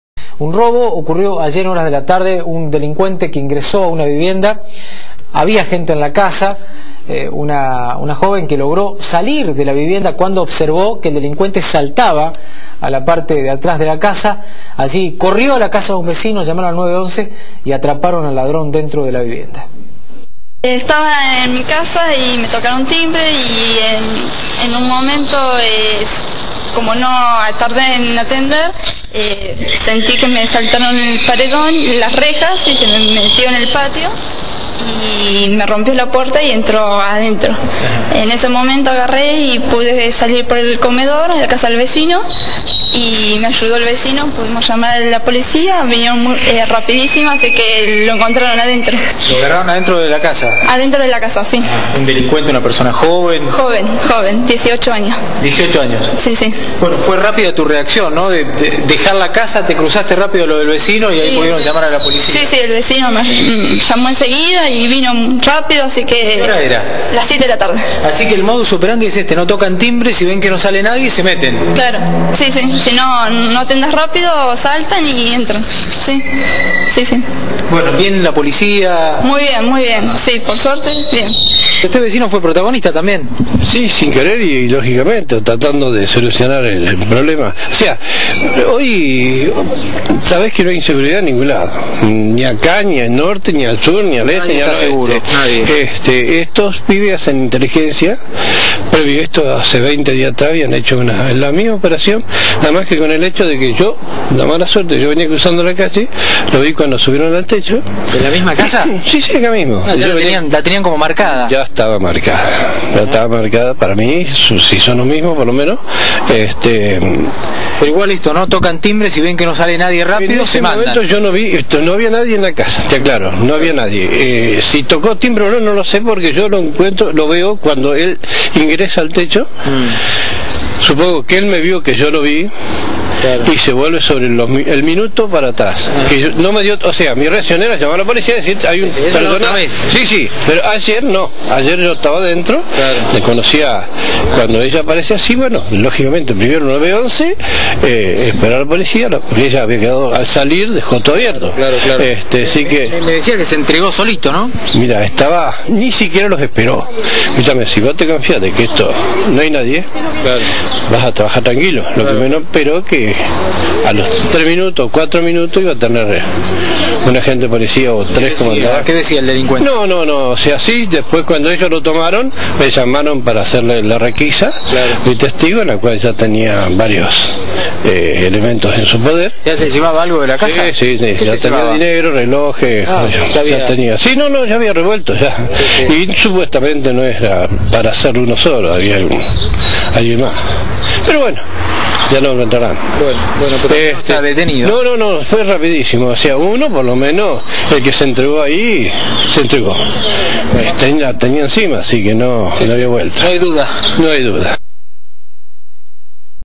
Atraparon a un ladrón dentro de una casa
El hecho ocurrió ayer a las 19 horas, en Tierra del Fuego 45. La joven escuchó como rompian un vidrio y corrió hasta lo de un vecino. Desde ahí alertó a los uniformados, quienes de inmediato llegaron al lugar.
Comentá esta nota